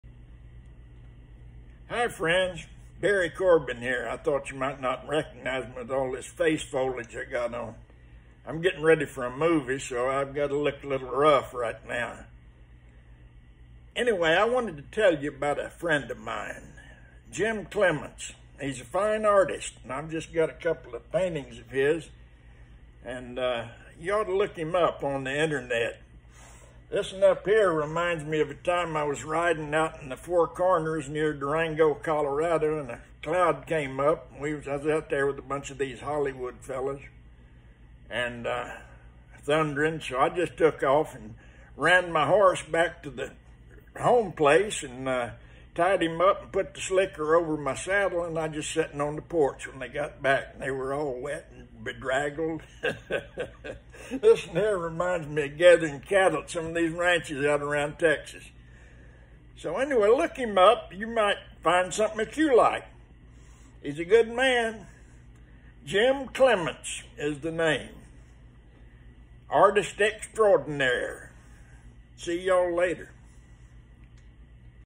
Actor Barry Corbin
Award-winning actor and Emmy nominee Barry Corbin took a moment to explain why he connects to my paintings.
He has starred in popular screen favorites like Urban Cowboy, Lonesome Dove and Conagher. He's an awesome guy I'm proud to call my friend. Be sure to check more paintings out on my